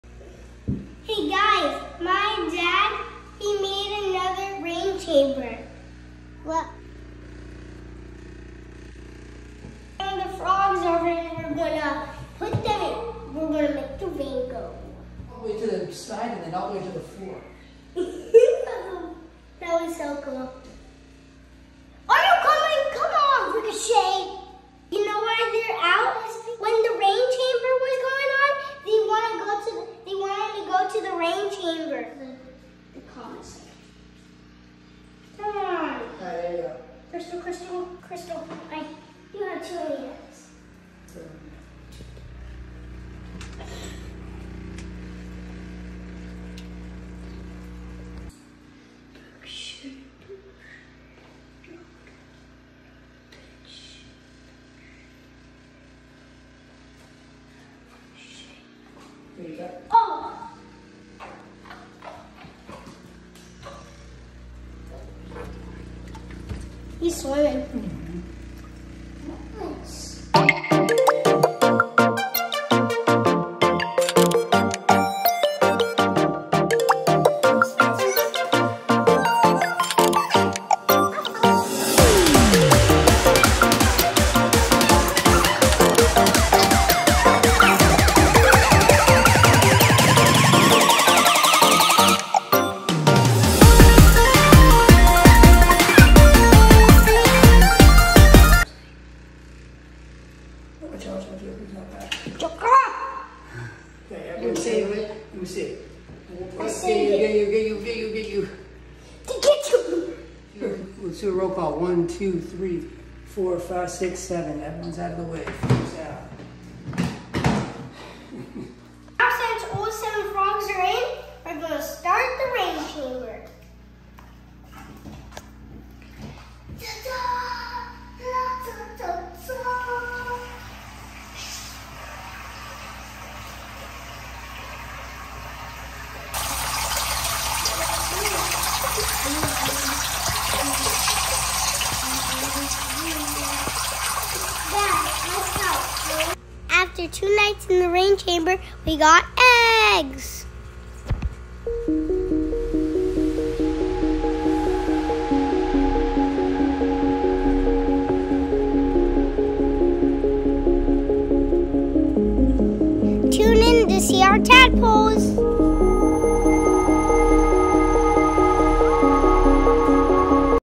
Rain Chamber for our Dumpy tree frogs!
New rain chamber for our dumpy tree frogs! We are trying to get tadpoles , hopefully this time we are successful.